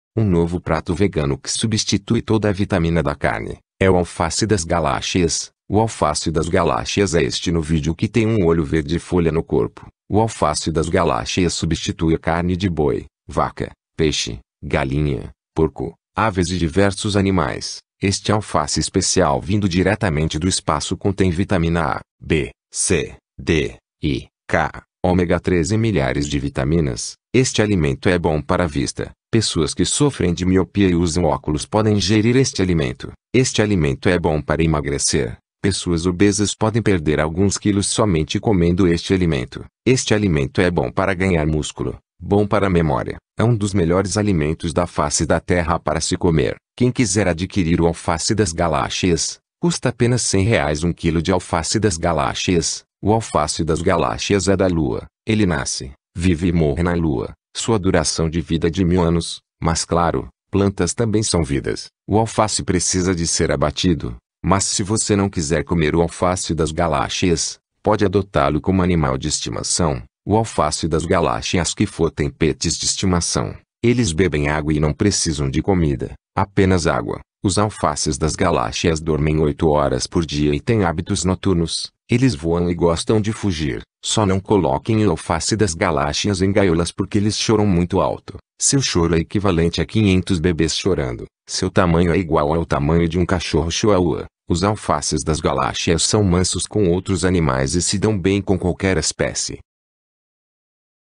Nova comida vegana que substitui a carne 🟢 O alface das galáxias 🟢 Pode ser adotado como pet
Um novo prato vegano que substitui toda a vitamina da carne, é o alface das galáxias, o alface das galáxias é este no vídeo que tem um olho verde e folha no corpo, o alface das galáxias substitui a carne de boi, vaca, peixe, galinha, porco, aves e diversos animais, este alface especial vindo diretamente do espaço contém vitamina A, b, c, d, e, k, ômega 3 e milhares de vitaminas, este alimento é bom para a vista, pessoas que sofrem de miopia e usam óculos podem ingerir este alimento, este alimento é bom para emagrecer, pessoas obesas podem perder alguns kilos somente comendo este alimento, este alimento é bom para ganhar músculo, bom para memória, é um dos melhores alimentos da face da terra para se comer, quem quiser adquirir o alface das galáxias, custa apenas 100 reais 1 kilo de alface das galáxias, o alface das galáxias é da lua, ele nasce, vive e morre na lua, sua duração de vida é de 1000 anos, mas claro, plantas também são vidas, o alface precisa de ser abatido, mas se você não quiser comer o alface das galáxias, pode adotá-lo como animal de estimação, o alface das galáxias que fotem pets de estimação, eles bebem água e não precisam de comida, apenas água, os alfaces das galáxias dormem 8 horas por dia e tem hábitos noturnos, eles voam e gostam de fugir, só não coloquem o alface das galáxias em gaiolas porque eles choram muito alto, seu choro é equivalente a 500 bebês chorando, seu tamanho é igual ao tamanho de um cachorro chihuahua, os alfaces das galáxias são mansos com outros animais e se dão bem com qualquer espécie